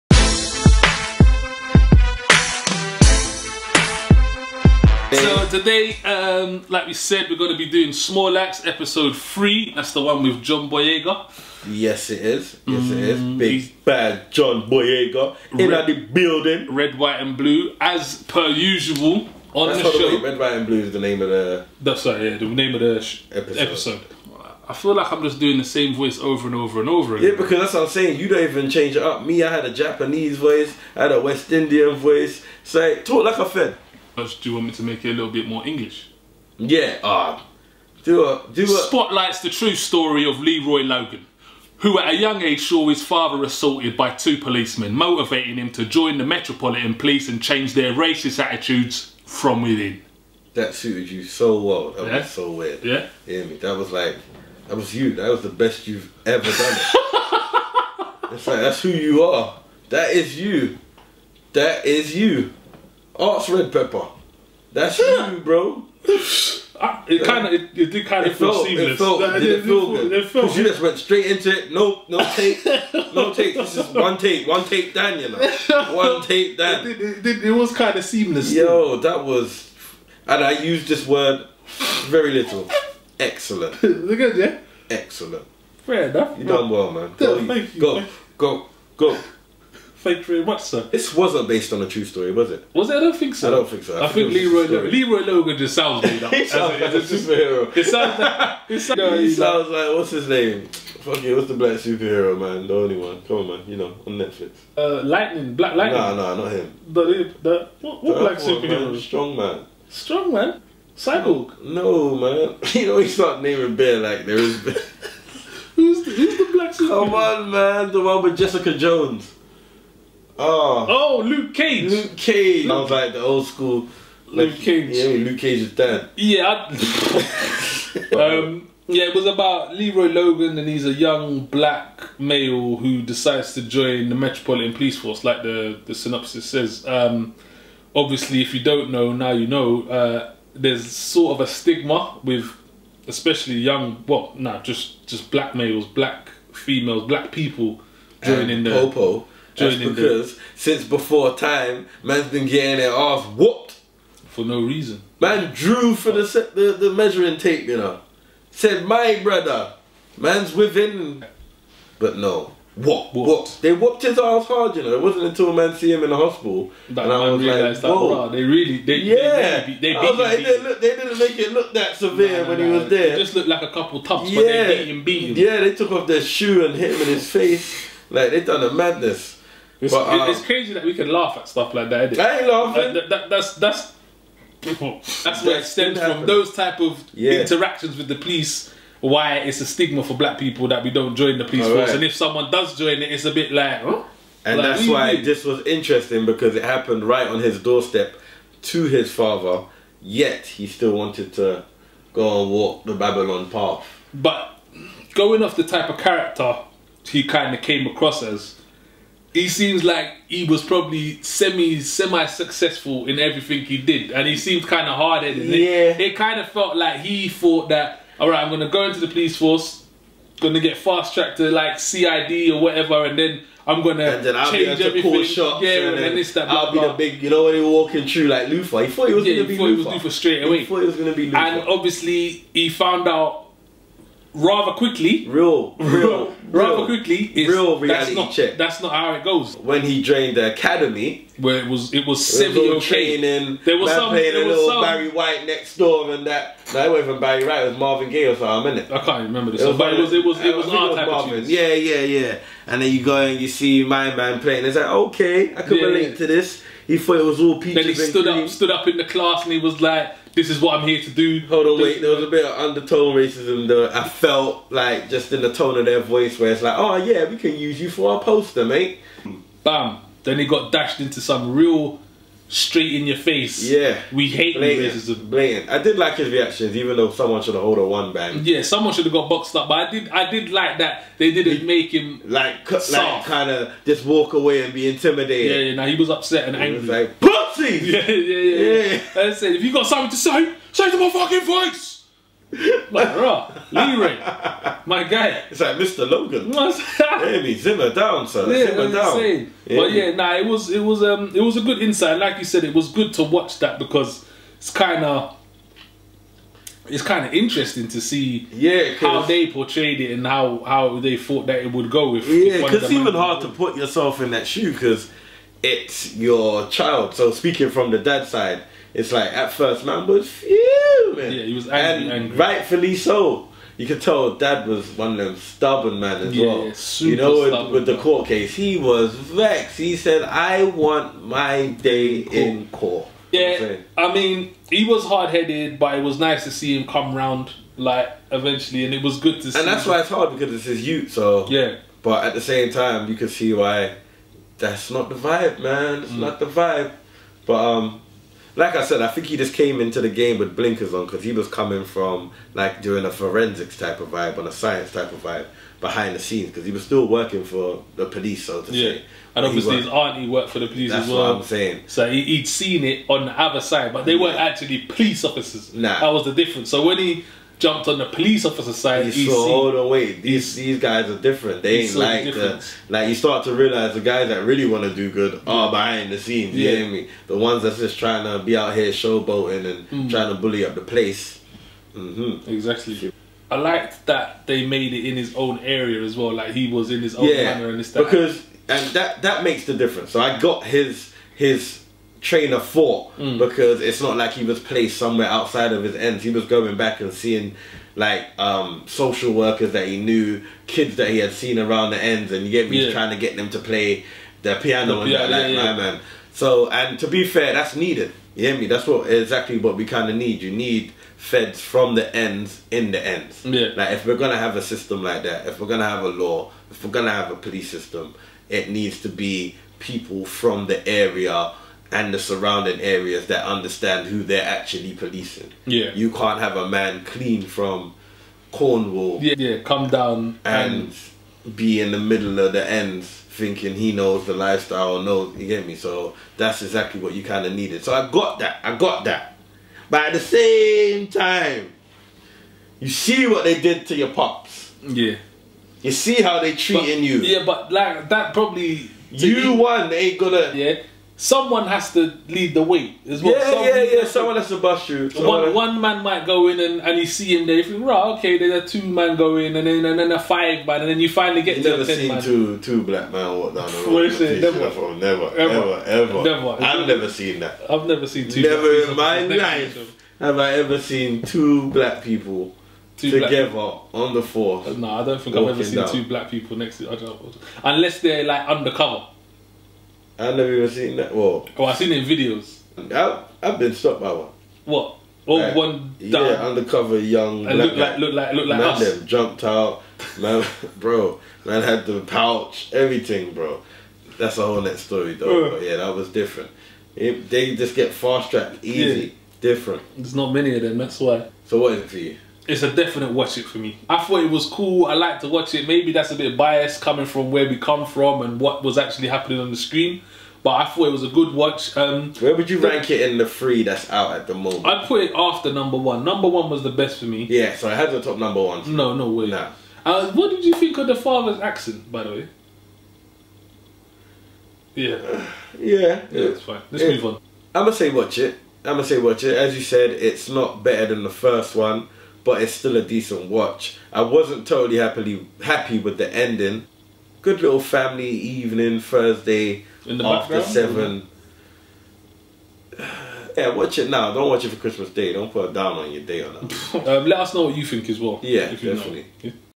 Watch It Or Not - Small Axe Episode 3 Red, White and Blue Review
Brand new  movie, TV show and anime review show with hosts Daniel Nero and Zush.
Today the guys review Small Axe episode 3 of the 5 part series from Steve Mcqueen.